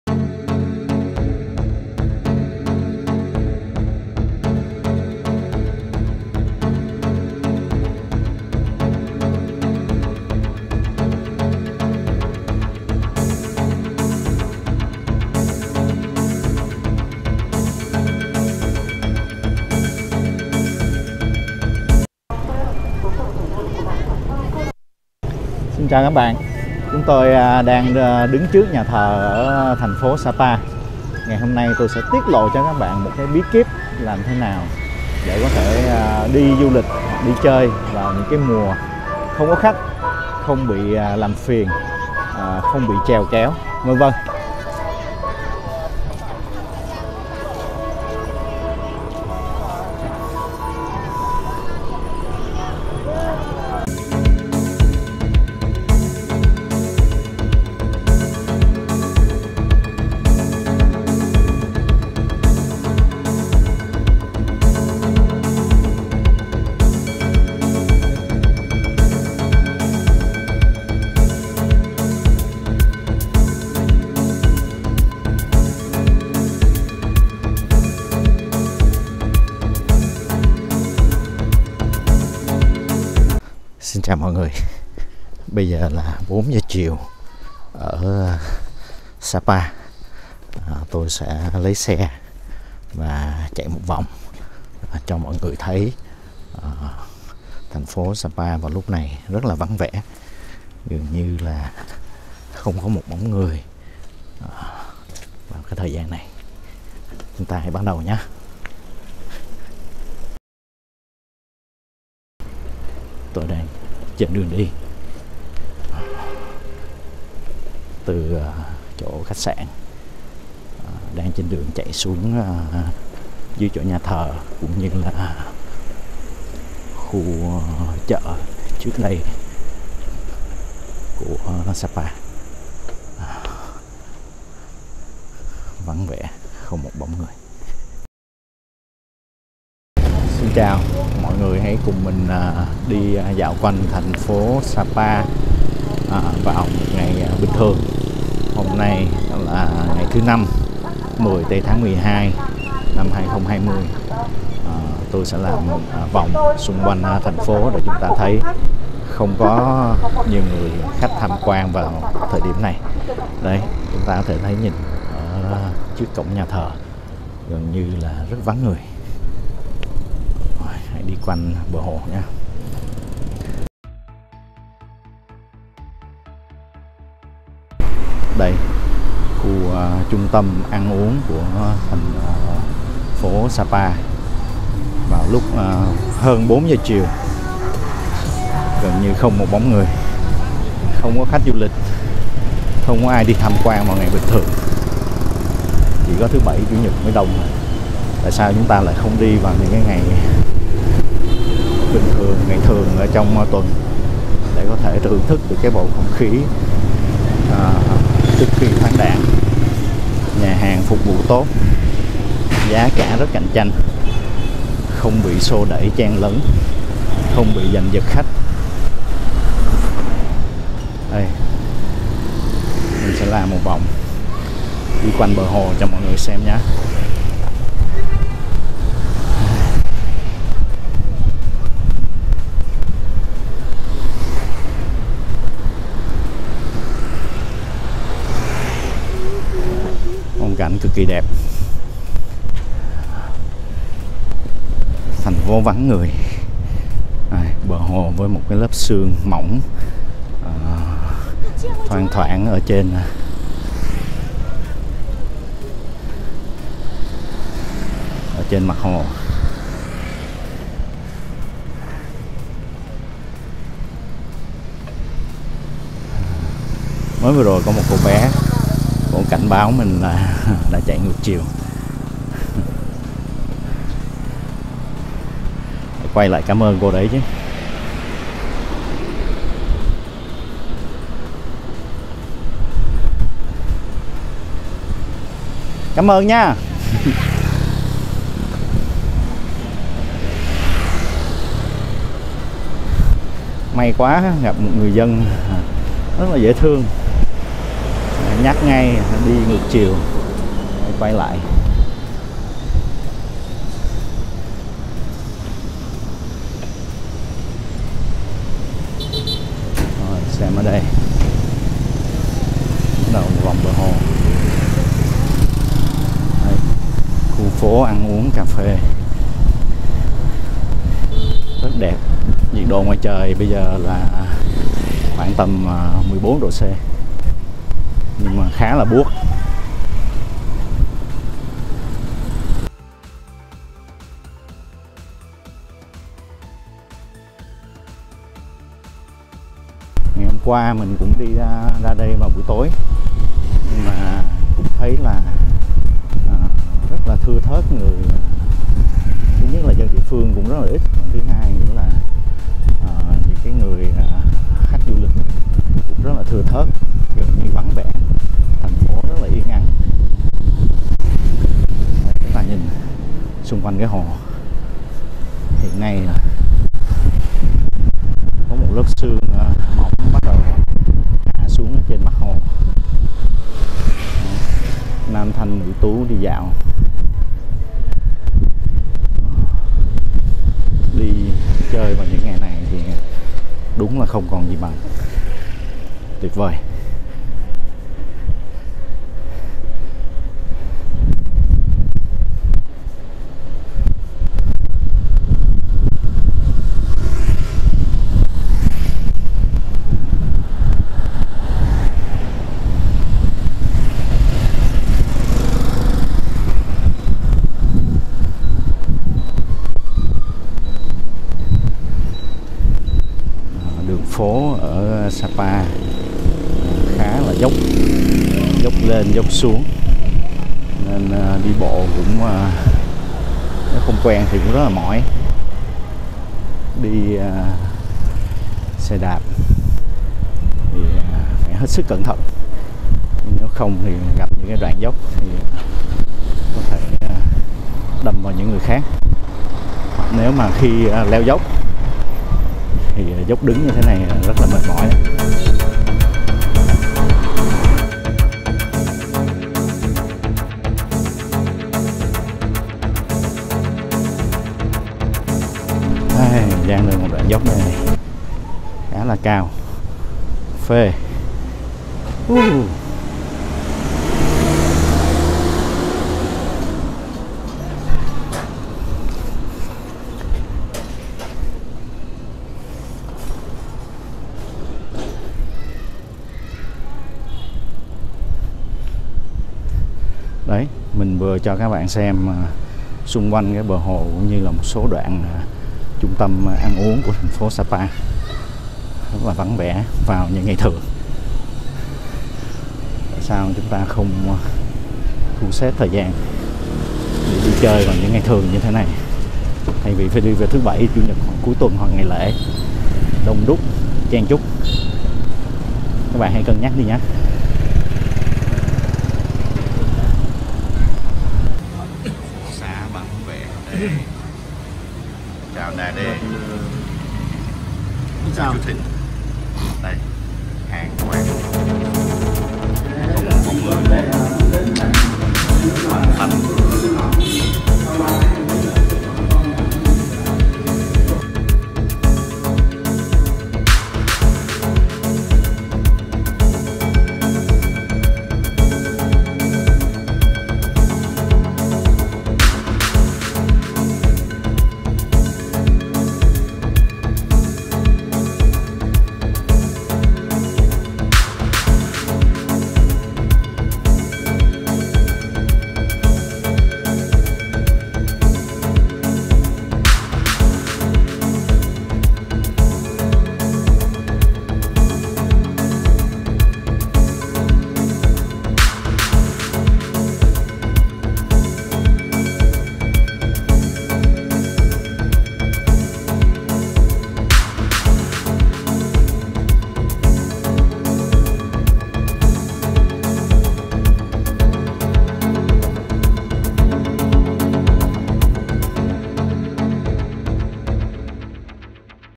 Tại Sao Chúng Ta Nên Đi Sapa Ngay và Luôn (Tháng 12-2020).
Hãy cùng khám phá một Sapa không có khách du lịch, vắng vẻ, thơ mộng, đẹp đẽ, an toàn giữa tháng 12. Một cơ hội hiếm có để cảm nhận Sapa ở góc nhìn rất khác.

Anh Ba Quạu
Review Sapa
Du Lịch Tự Túc